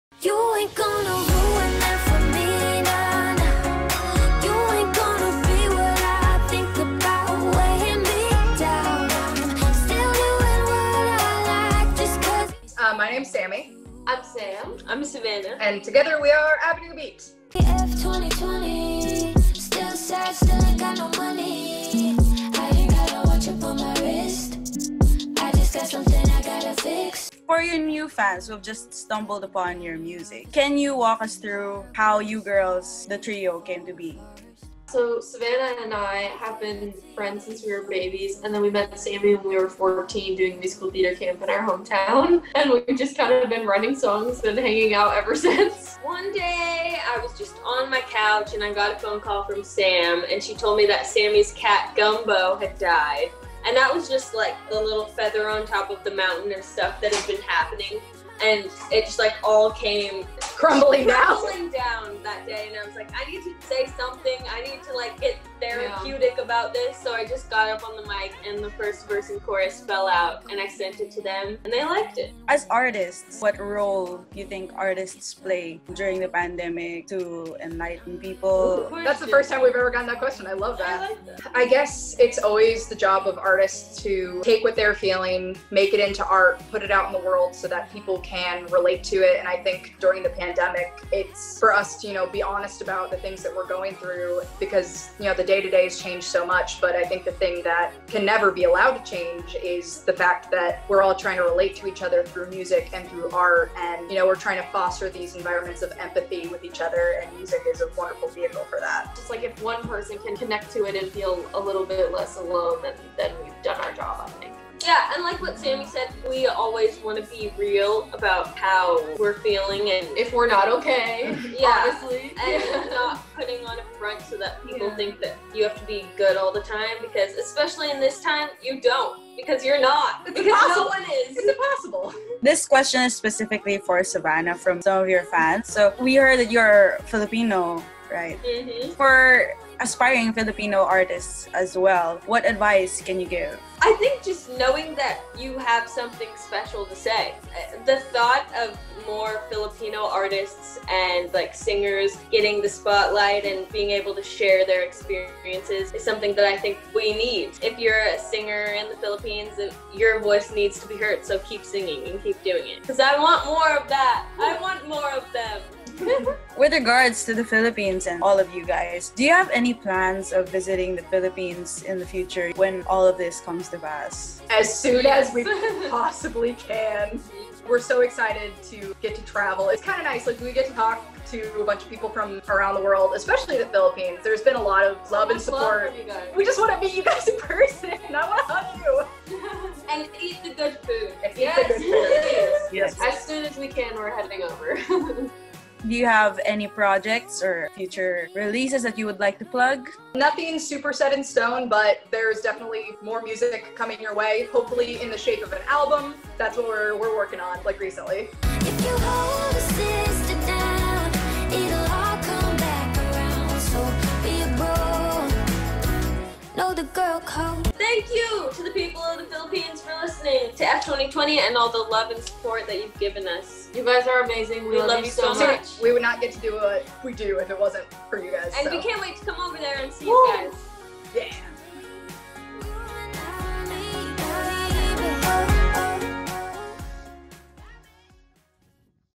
Get To Know AVENUE BEAT In This Exclusive Interview With uDOu
uDOu got to chat with American trio Avenue Beat where we talked about how they started, their music, and what for them is the artist's role during this pandemic.

Follow uDOu ACCOUNTS to keep updated on your favourite artists!